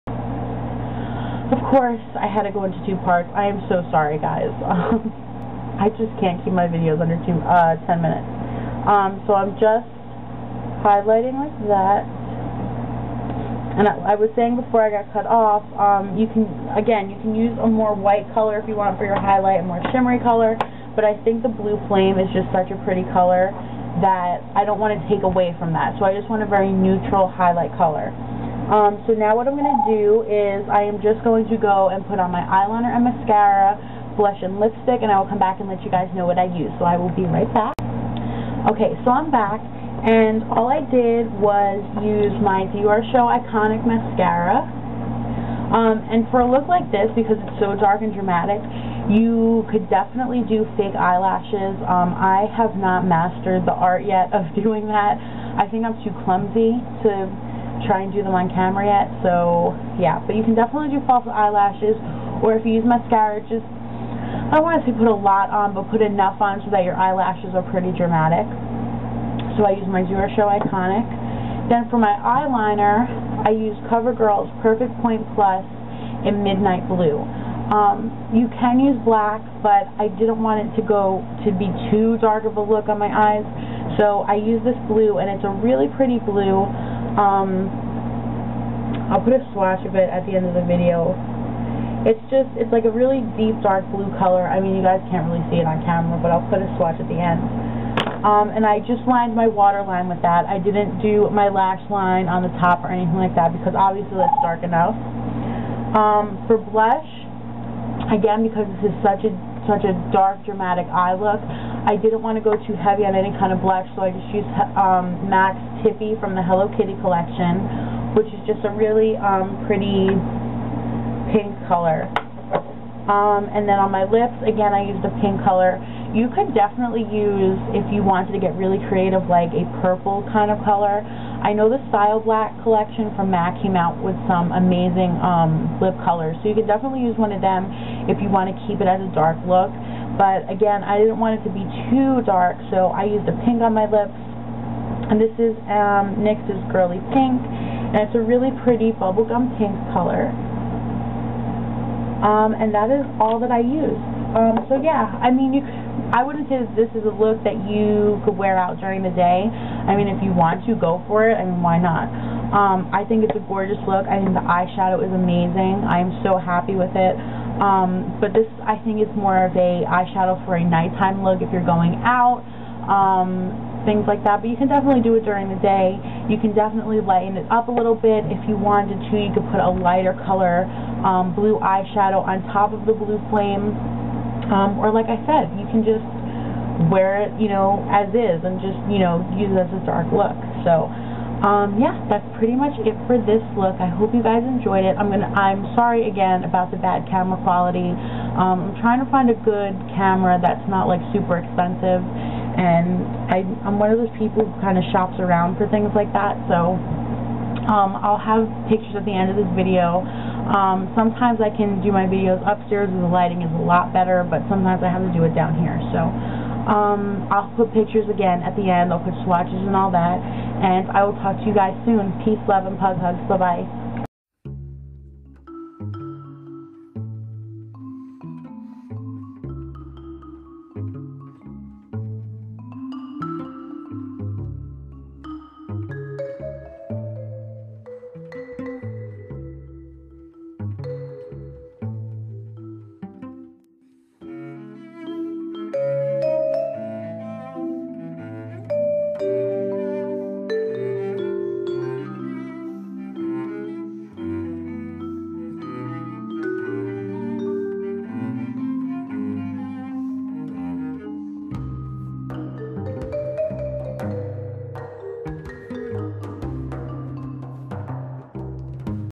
Tutorial using MAC "Blue Flame" Mineralize Eye Shadow PART 2
Part two of my tutorial using MAC's eyeshadow "Blue Flame" from the Style Black collection.
. He does AMAZING tutorials and I got the idea to use NYX's "Pacific" as a base from his "Blue Flame" tutorial!! He is absolutely fabulous!!
Products Used:
MAC Studio Sculpt Foundation NC20
MAC Studio Finish Concealer in NW20
MAC Mineralize Skinfinish Natural in "Medium Plus"
MAC "Painterly" Paintpot
NYX Jumbo Eyeshadow Pencil in "Pacific"
MAC Mineralize Eyeshadow in "Blue Flame"
NYX Jumbo Eyeshadow Pencil in "Baby Blue"
Rimmel London "Colour Rush" Quad (Beige color for highlight)
Cover Girl Perfection Point Plus Eyeliner in "Midnight Blue"
Diorshow Iconic Mascara
MAC Blush "Tippy"
NYX Lipstick in "Girly Pink"